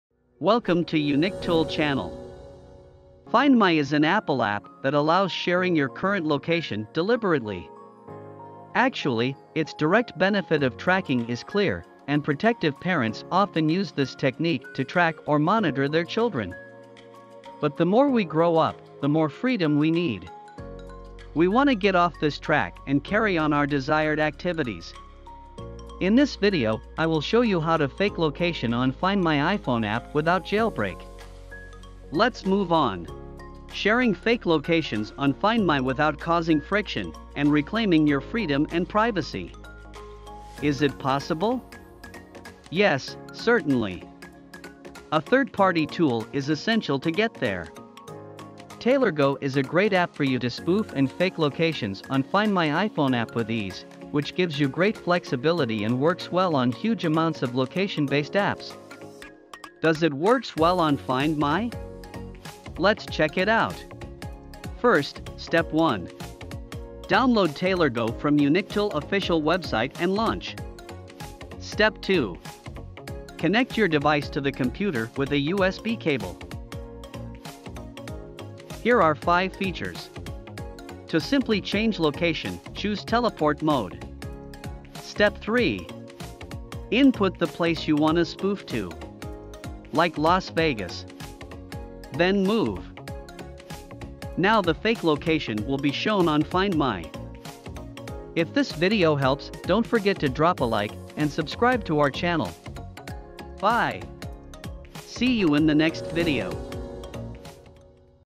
How to fake/spoof location on Find My iPhone - no Jailbreak
Do you wanna get rid of Find My tracking and carry on our desired activities? In this video, I will show you how to fake location on Find My iPhone app without jailbreak, let's move on!

Timestamp
00:00 Intro & preview
00:35 How to fake location on Find My, no Jailbreak
Step1. Download TailorGo from Unictool Official website and launch.
Step2. Connect your device to the computer with a USB cable.
Step3. choose Teleport mode, then Input the place you wanna spoof to.